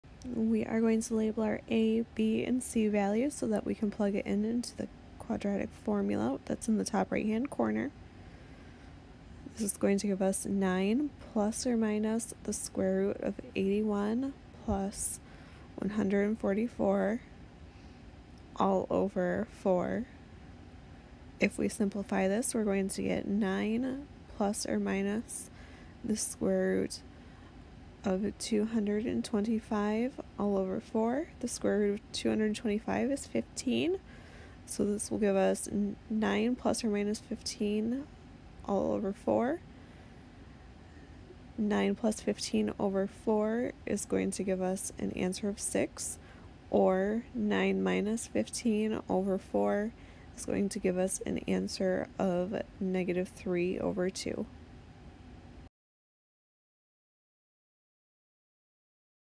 Quadratic Equation: Solve 2x^2 - 9x - 18 = 0
Tiala from SVSU Micro Math helps you solve a quadratic equation using the quadratic formula.  This equation has two distinct rational solutions.

Problem: Solve 2x^2 - 9x - 18 = 0